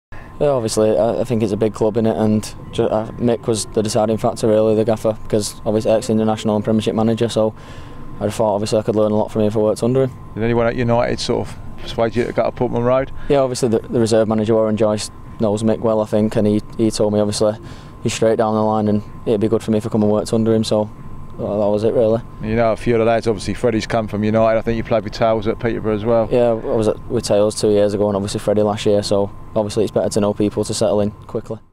Ryan Tunnicliffe: The first interview as a Blue
Ryan Tunnicliffe gives his first interview as an Ipswich Town player after joining on loan from Manchester United.